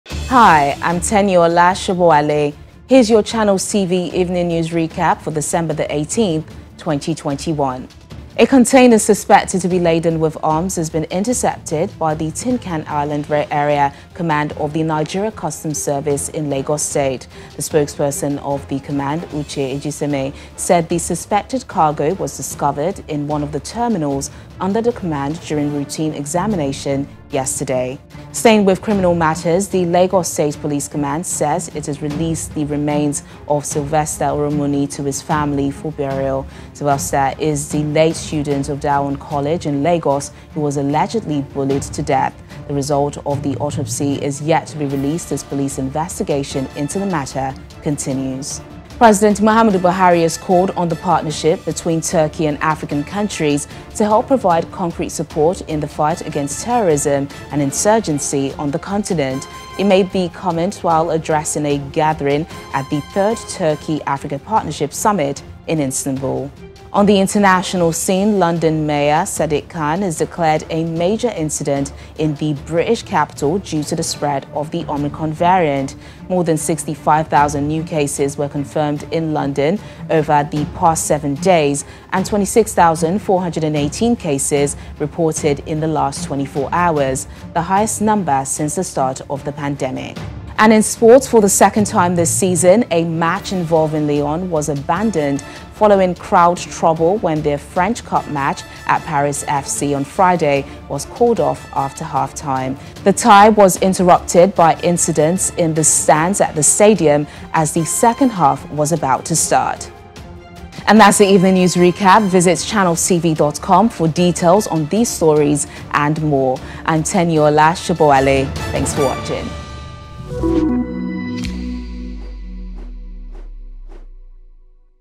Recap: Body Of Sylvester Oromoni Released To Family, Says Police
The Lagos state police command says it has released the remains of Sylvester Oromoni to his family for burial.

Sylvester is the late student of Dowen College in Lagos who was allegedly bullied to death.